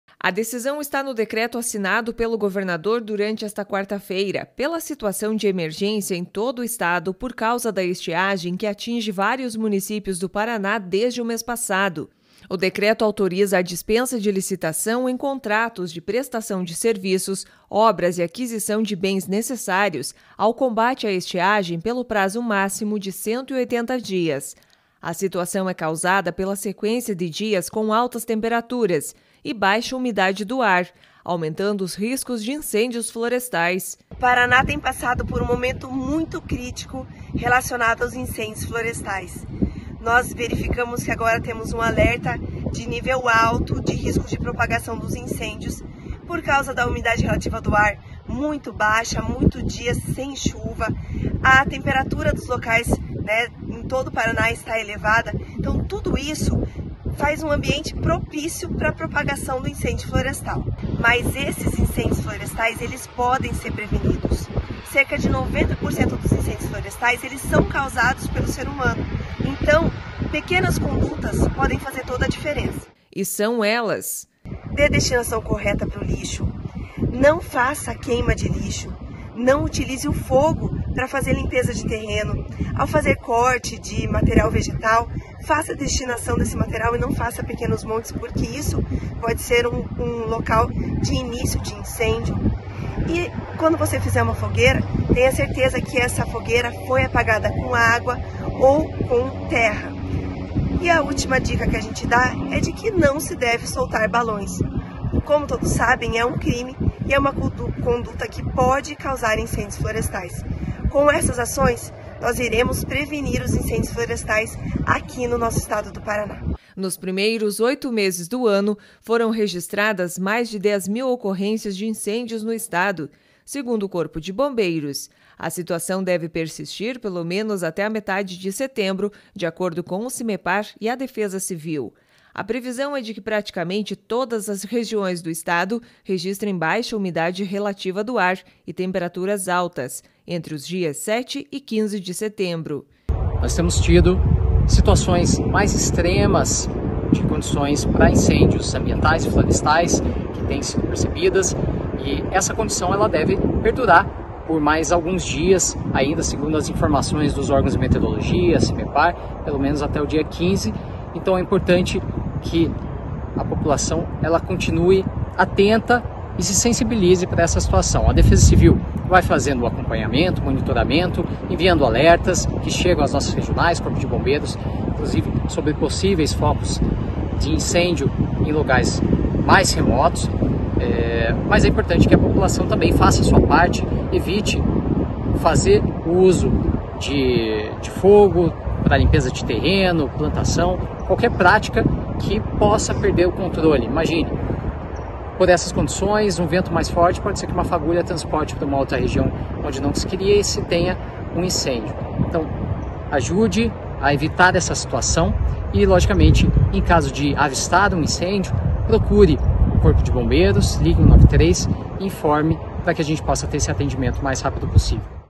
No Paraná, governador decreta emergência de estiagem
Meio Ambiente: Somente em agosto, o Estado registrou cerca de 20 mil focos de calor, aproximadamente seis vezes mais do que foi registrado em julho. Governador do estado decreta emergência de estiagem.